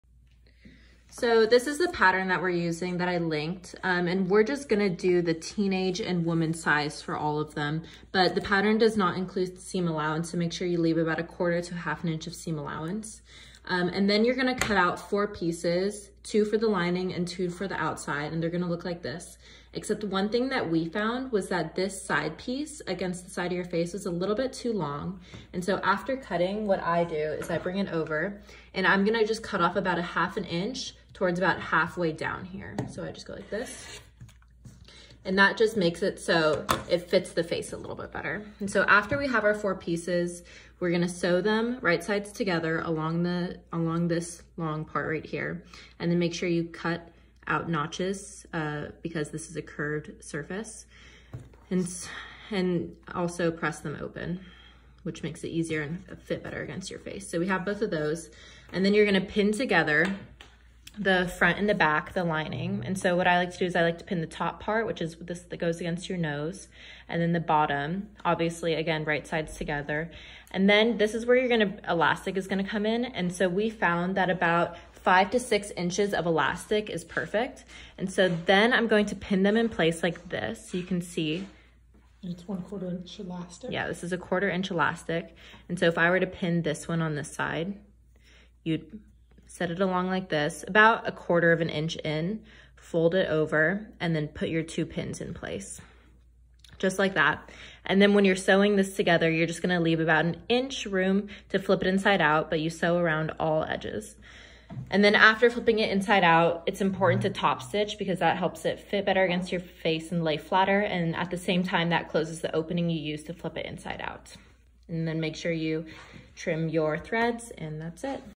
How to Sew your own Face Masks!
This is how we are sewing face masks for non-clinical workers in hospitals. The pattern will be linked below.

Some essential information: this is an easy project that can utilize leftover fabric (cotton and flannel only) and quarter inch flat elastic or round 1/16 round elastic.

IMPORTANT: elastic may be hard to get, ties that go around the head can work also!! See the link for an example:

Joann’s is still doing curbside delivery for call in fabric orders and you can order the elastic on Amazon. Hospitals have asked for “thousands of masks”.

Elastic that would work: